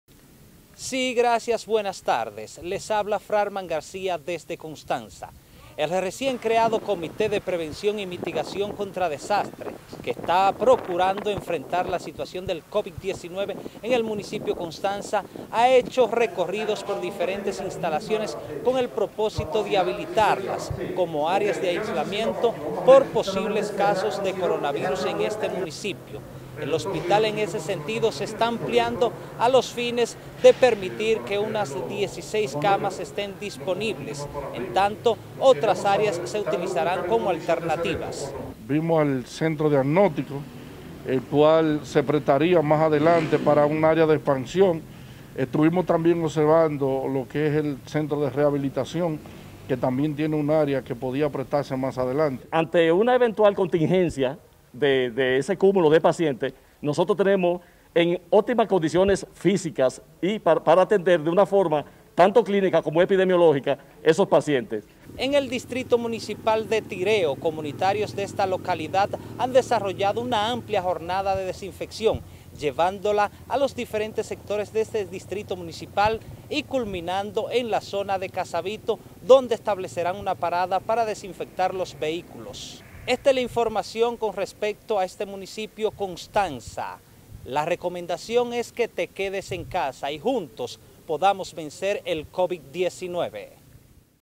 Realizan jornada de desinfección en Constanza
Un recorrido por otras provincias del país donde la aglomeración de personas sigue siendo el denominador común, mientras autoridades continúan tomando medidas, iniciamos en La Altagracia.